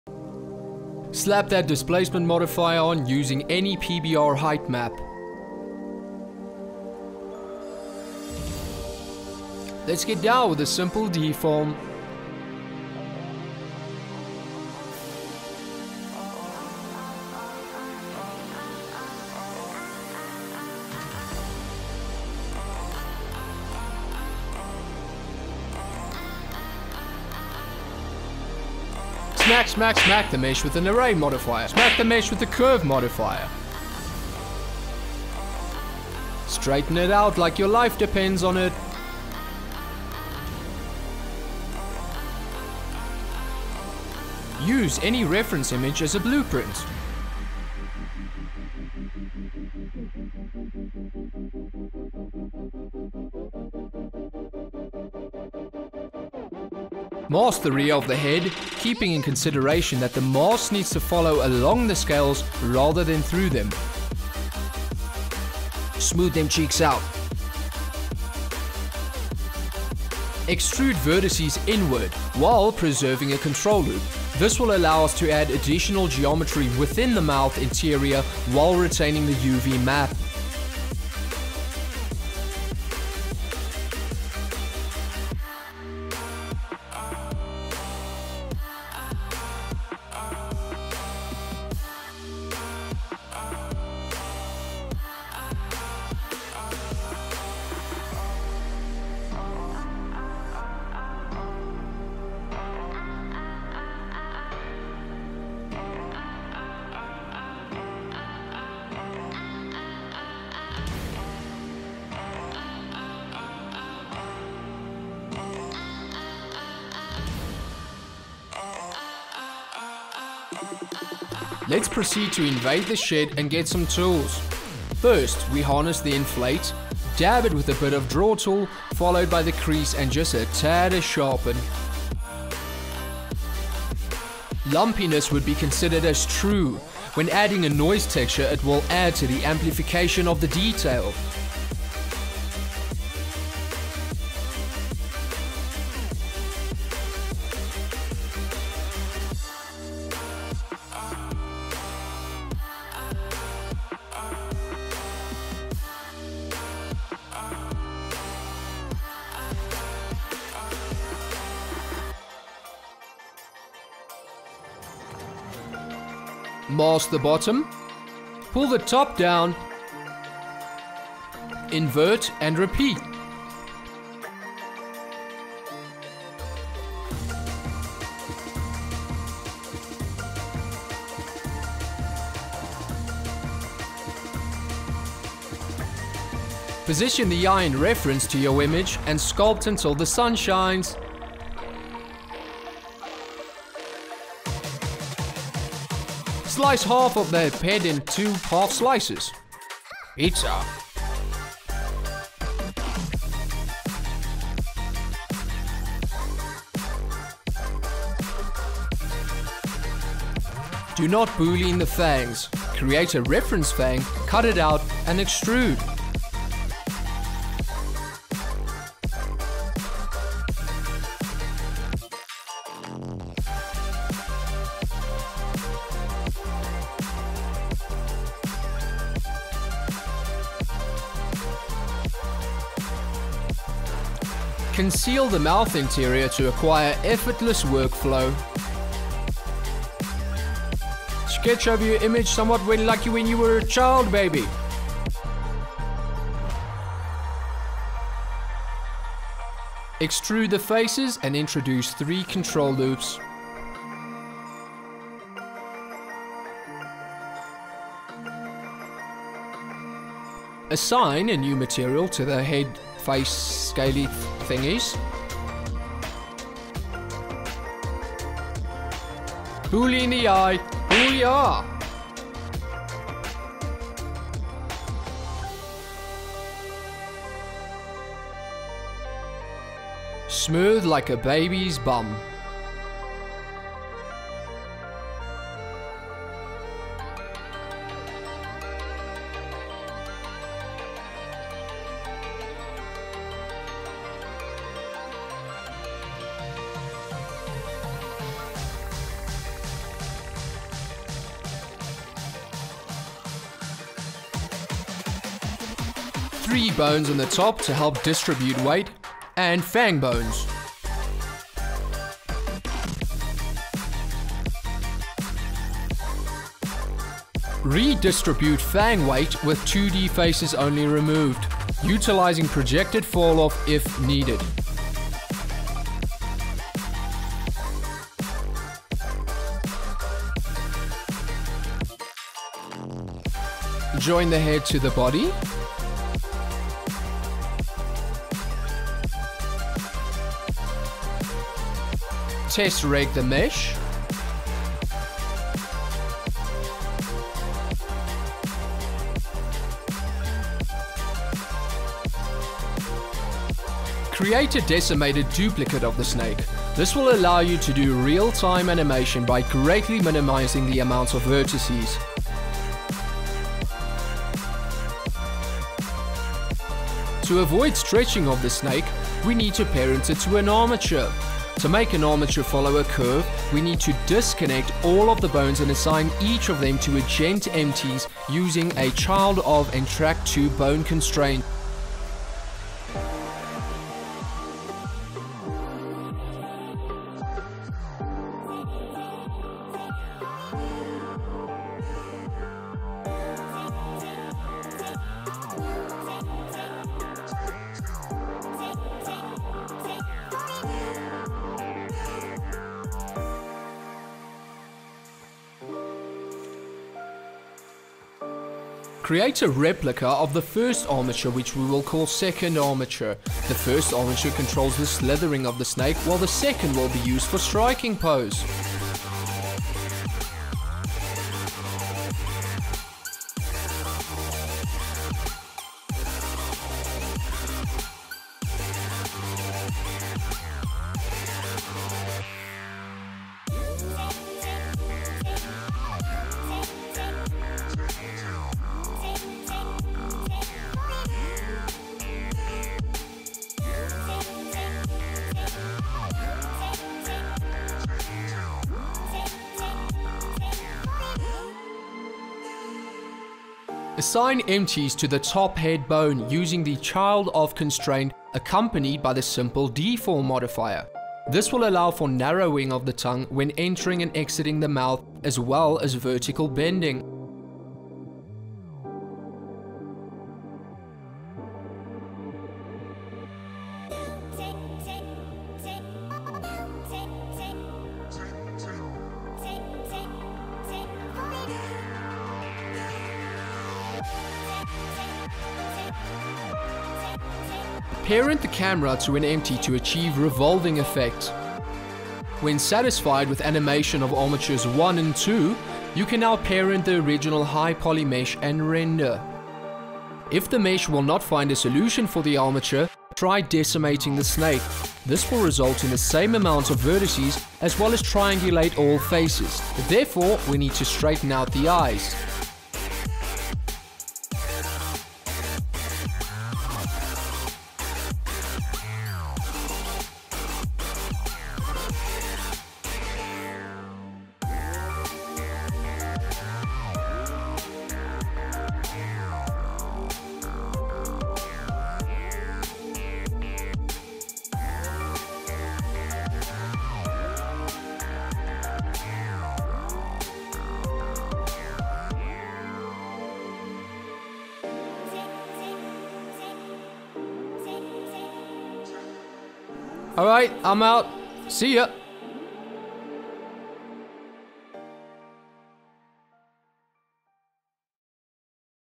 How To Snake In 9 Minutes - Blender Tutorial
I Had 1 Job - Don't delete the original footage

Oh well..

                                                   [  Snake Models For Download  ]

0:00 Modelling
05:00 Rigging
06:47 Animation